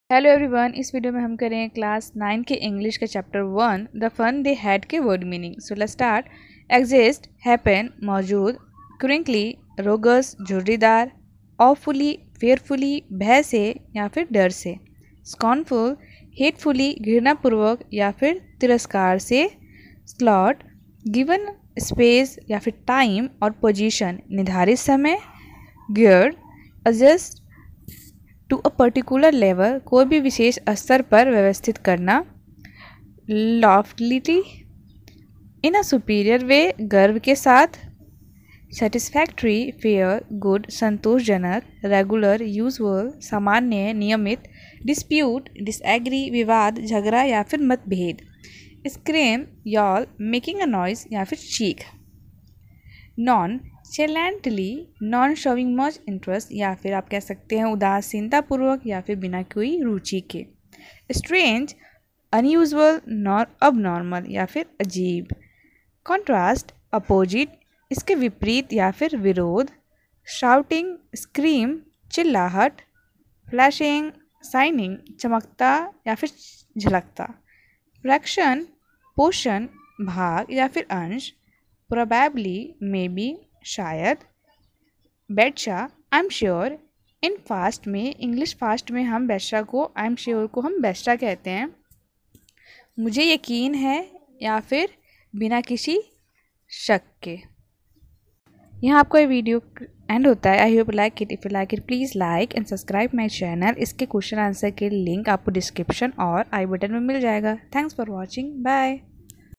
Class 9 English the fun they had word meaning|| ncert solutions for class 9 english chapter 1
Class 9 English the fun they had word meaning
NCERT solutions for class 9 English Chapter 1

Hi Everyone,
This video is about class 9 english the fun they had word meaning. In this video I have explained all important word meaning of ncert solutions for class 9 english chapter 1.
I hope you like this explanation.

class 9 english the fun they had question answer